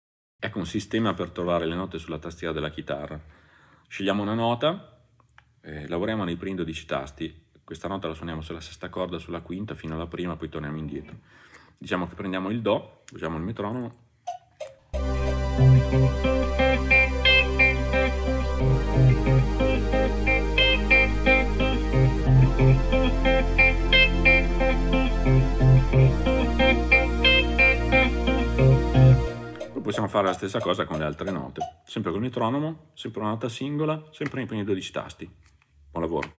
Chitarra: le note sulla tastiera
Ecco un piccolo tutorial per imparare le note sulla tastiera della chitarra. Buon lavoro!
Learn the notes on the fretboard with this little exercise.

This means you can use this music and/or video for noncommercial use, with a credit and a link to the original version.